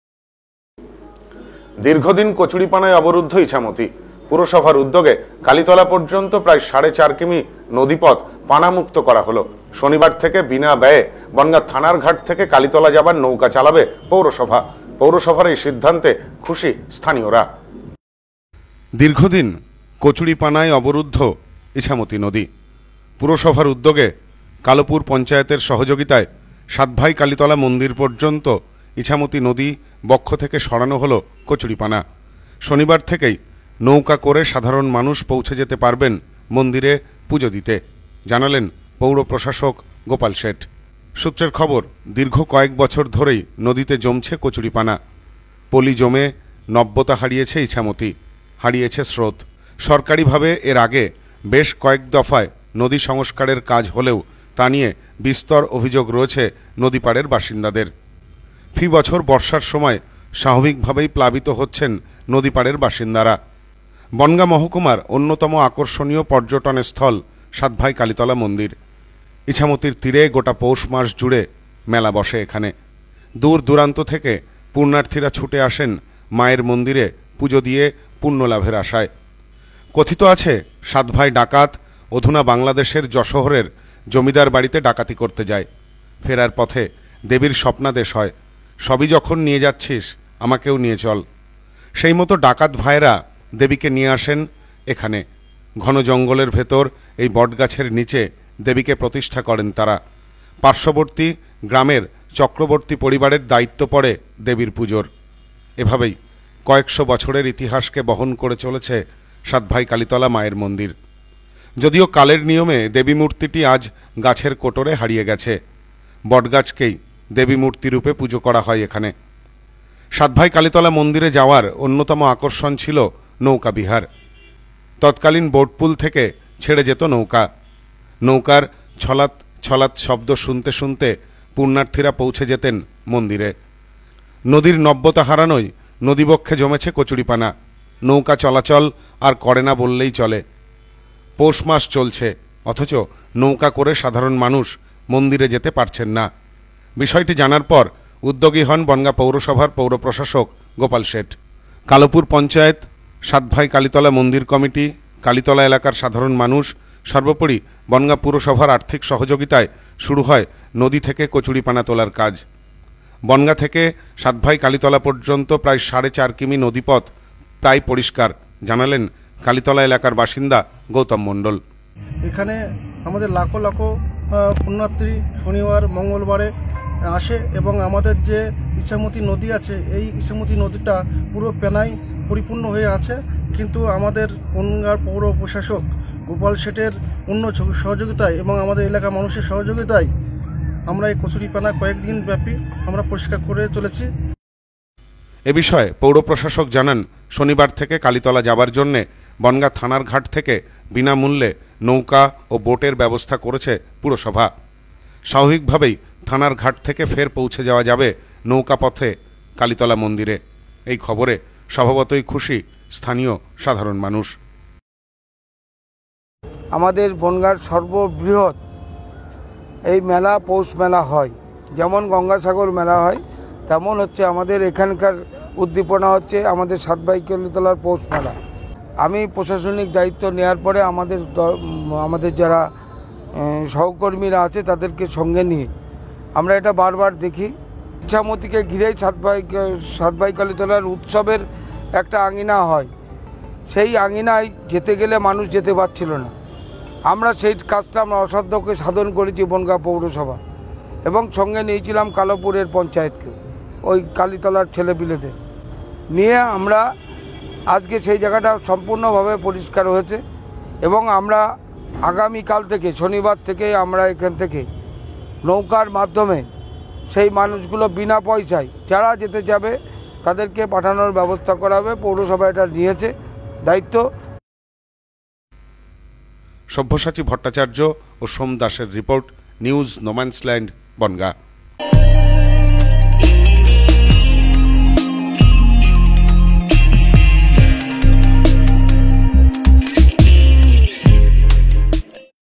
কচুরিপানা সাফ, পৌরসভার উদ্যোগে বিনা ব্যয়ে নৌকা যাবে কালীতলায়।
দীর্ঘদিন কচুরিপানায় অবরুদ্ধ ইছামতি। পুরসভার উদ্যোগে কালিতলা পর্যন্ত প্রায় সাড়ে চার কিলোমিটার নদী পথ পানা মুক্ত হলো। শনিবার থেকে বিনা ব্যায়ে থানার ঘাট থেকে কালিতলা যাবার নৌকা চালাবে পুরসভা। খুশি স্থানীয়রা।